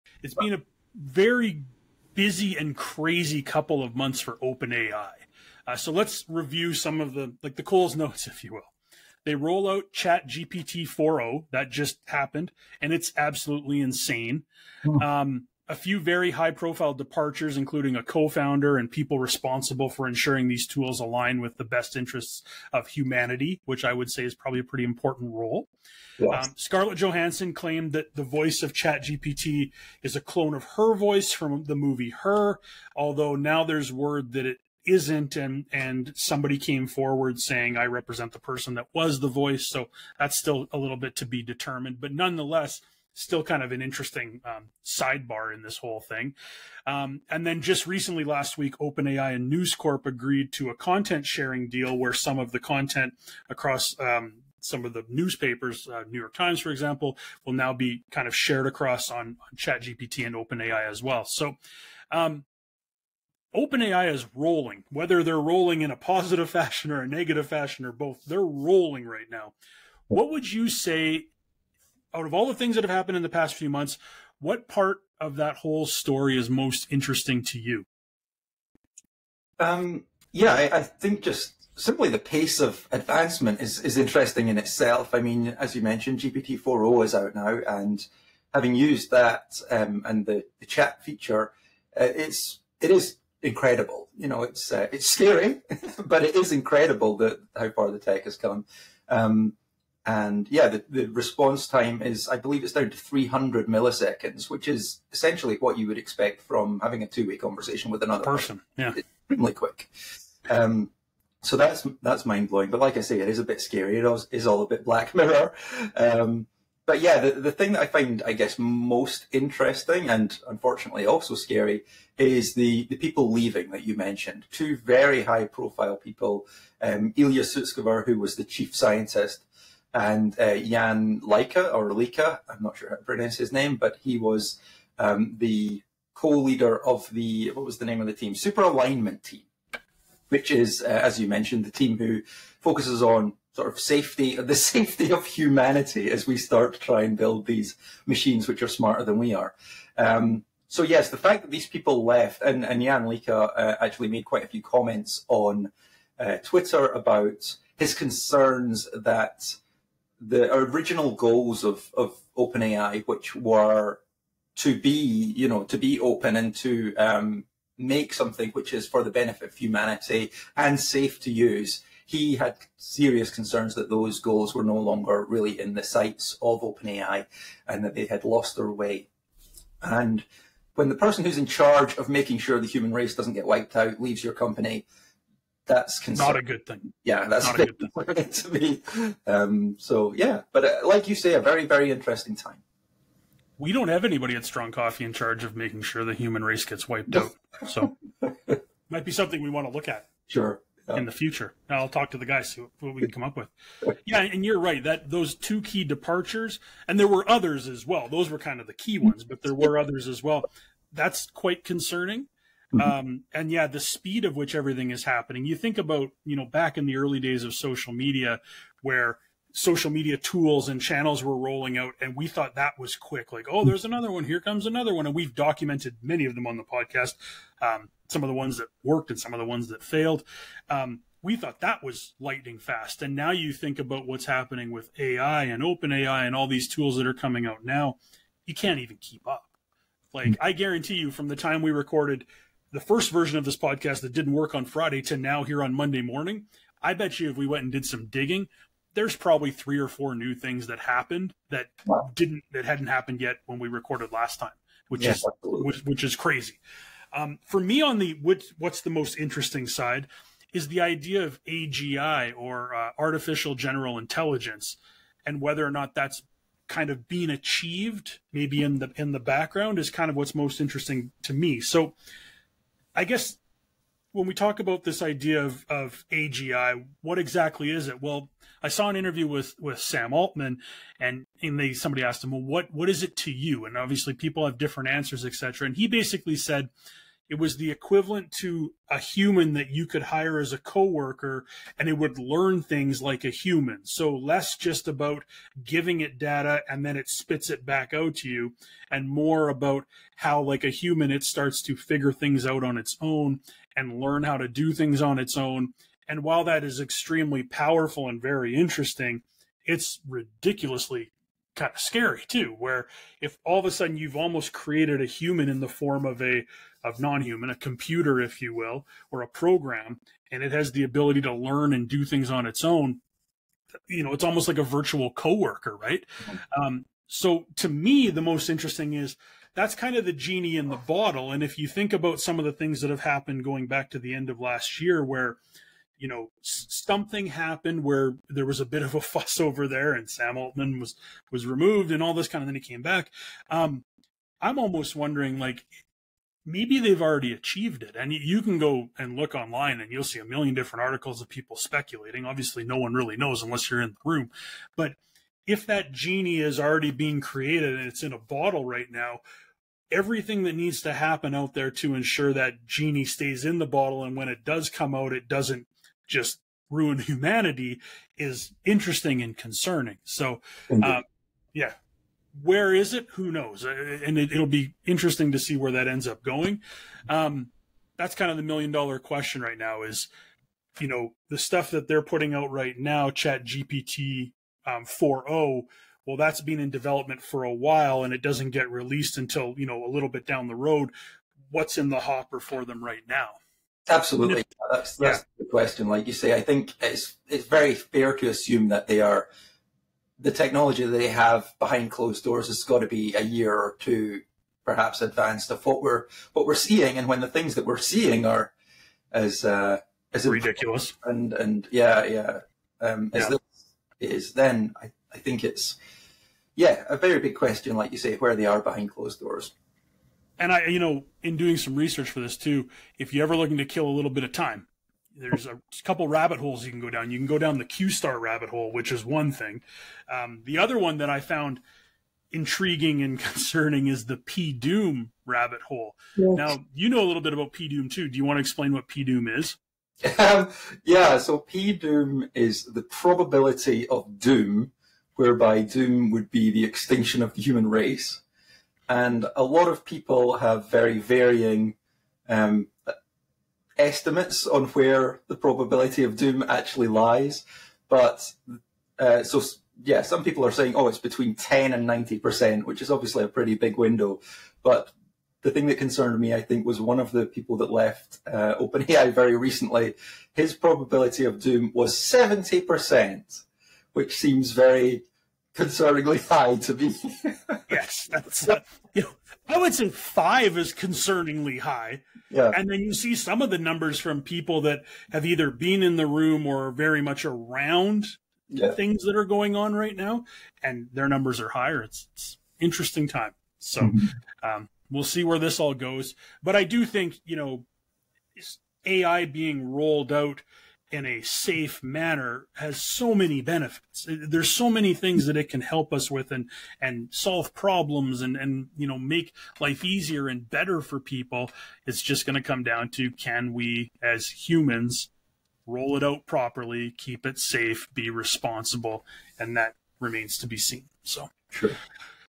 Lots Happening at OpenAI - ChatGPT 4.o Arrives!
It has been a pretty crazy couple of months for OpenAI. Let’s review…

They roll out GPT-4o (ohh), and it’s insane!

A few very high-profile departures (including a co-founder)  - people responsible for ensuring these tools align with the best interests of humans

Scarlett Johansson claimed that the voice of ChatGPT is a clone of her voice from the movie “Her” (Now there’s word that it isn’t Scarlett Johansson and is instead a different actress)

OpenAI and News Corp agree to a content-sharing deal

Rumors of a major breakthrough with regards to AGI (Artificial General Intelligence)

OpenAI is rolling right now. What part of their past few months is most interesting to us?

P.S. We also touch on the P(doom) rabbit hole!